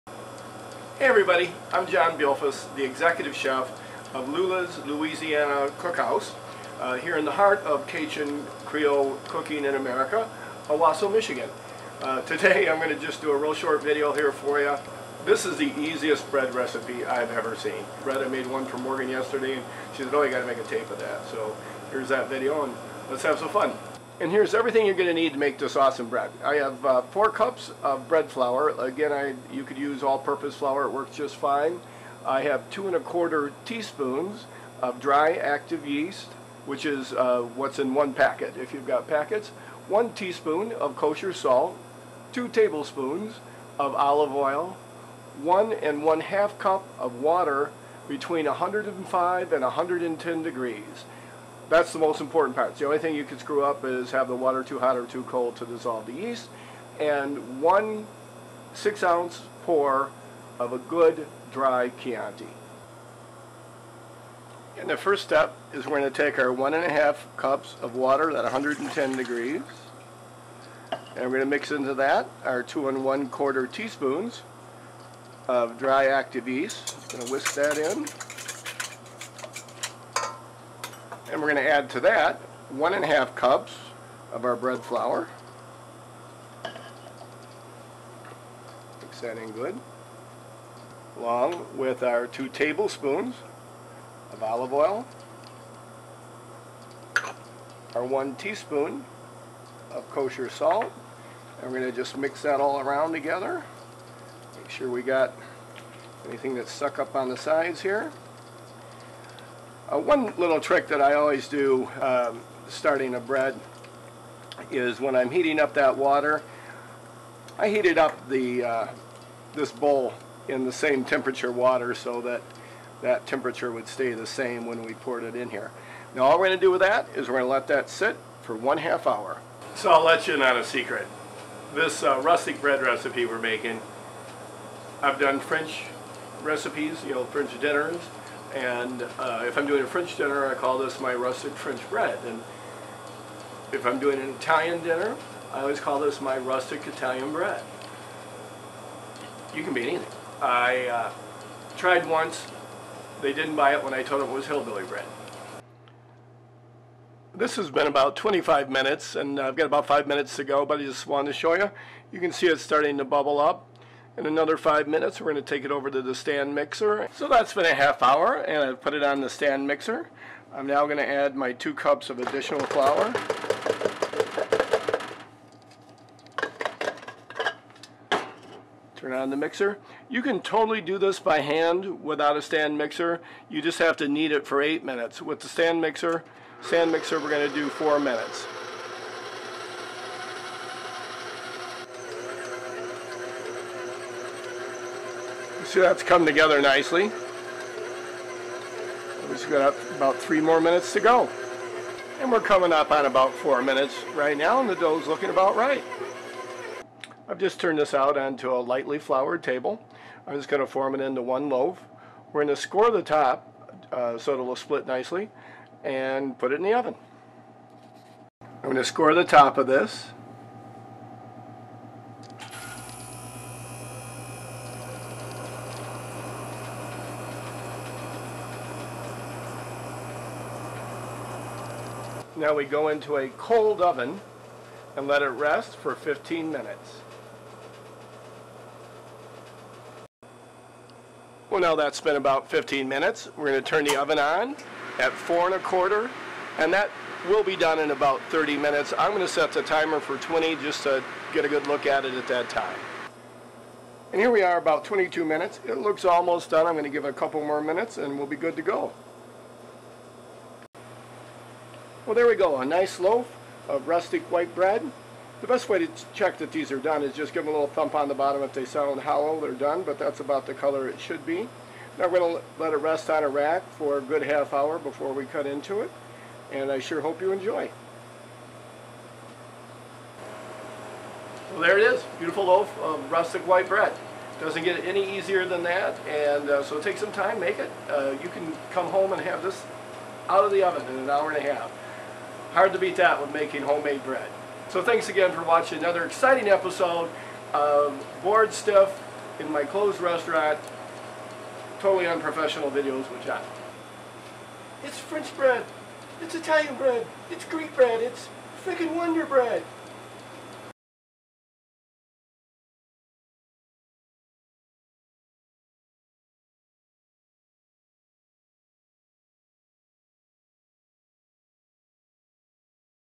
How to make Easy Rustic Bread with John Beilfuss
John Beilfuss, Executive Chef of Lula's Louisiana Cookhouse, shares an easy Rustic Bread recipe while sheltering in place!